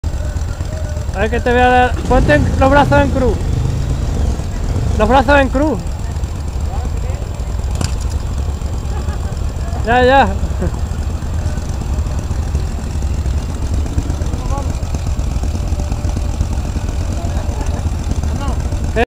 Intermitencias y Luz de Freno extras para la moto.
Esto es un "invento-Chorrada" que he hecho para la moto. Por qué? Porque me aburro mucho. El caso es que son unas intermitencias que se llevan en los brazos. Y una luz de freno, en la espalda. Está conectado a la moto y hay que enchufarlo cada vez que subes y bajas. Pero ya tengo ideada la forma de hacerlo inalámbrico. Lo he hecho con velcro para que se pueda poner y quitar en una chaqueta u otra. En este vídeo lo lleva puesto mi hermana. Que la tuve que convencer porque sice que soy un "frikie" por hacer estas chorradas!! ;)